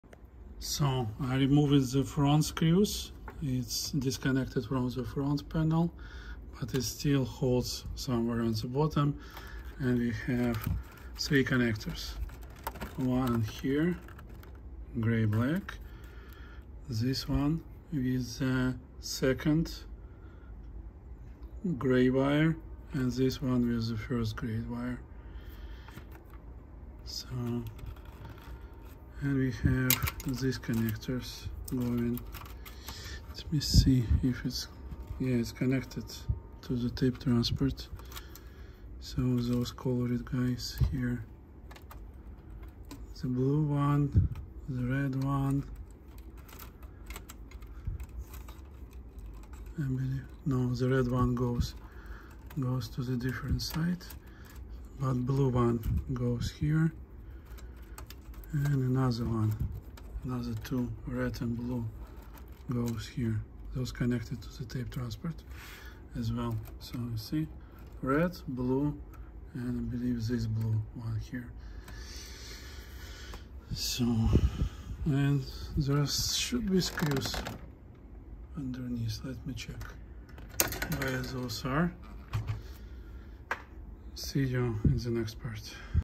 Pioneer CT-91a Restoration Part 3: Navigating the Electrical Connectors to the Tape Transport
Welcome to Part 3 of our Pioneer CT-91a Restoration series! This time we are delving into the electrical side of things, specifically the connectors that link the tape transport mechanism to the rest of the deck.

The electrical connectors in a cassette deck play a vital role in ensuring the proper operation of the device. They transmit signals between different components, including the tape transport, enabling these parts to work together in harmony. In the Pioneer CT-91a, these connectors are intricately designed and require careful handling during restoration.

In this video, I'll show you how to locate, inspect, and safely disconnect these connectors. This information is crucial if you're planning on removing the tape transport for servicing, or if you need to troubleshoot any electrical issues with the deck.

Join me in this detailed walkthrough as we continue to bring this iconic Pioneer deck back to life. Your likes, shares, and comments are always appreciated.